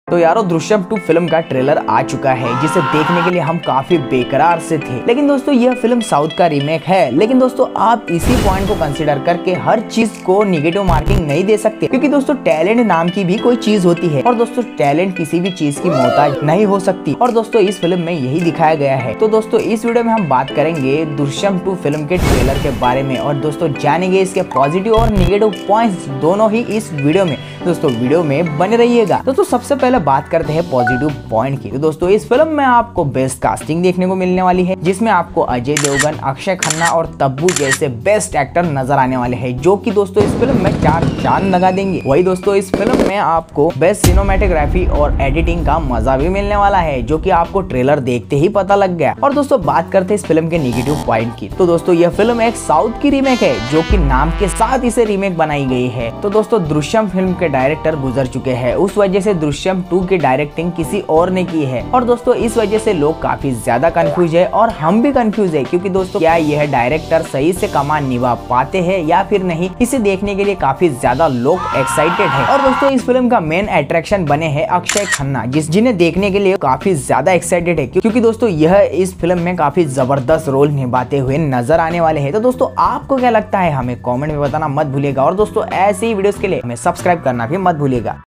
दृष्यम 2 फिल्म ट्रेलर review | drishyam 2 trailer review in hindi | ajay devgan, akshay khanna
in this video me ham bat karenge ajay devgan aur akshay khanna star drishyam 2 movie trailer ke bareme aur karenge uska review

in thise video we discuss about drishyam 2 movie trailer and review it so video watch till end
                                           team notify